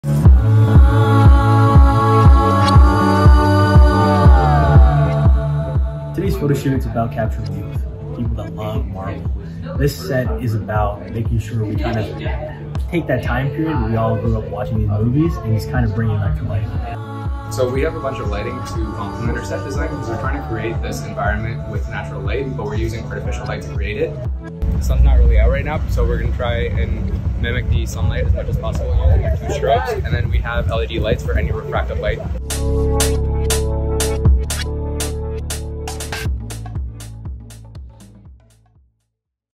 DIM MAK: The Direction s5e2
Behind the scenes looks at the DIM MAK: Avengers photoshoot.